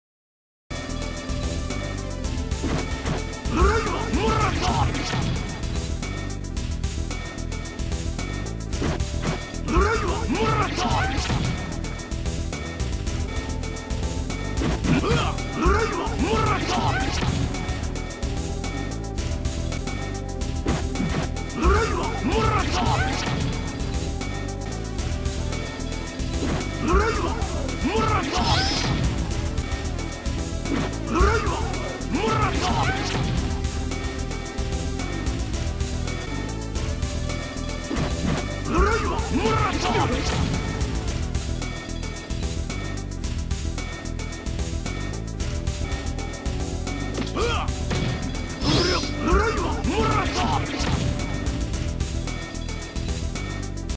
Bishamon ES unblockable setups
Here some setups you can do with bishamon, i think that some setups are too slow, but cr.lp/lk,cr.mp- ES Spririt works very well in real matches, especially vs opponents who like to block a lot

note that most of the setups have some variants (s.lp instead cr.lp and others)

Tested on the PS2 version and Arcade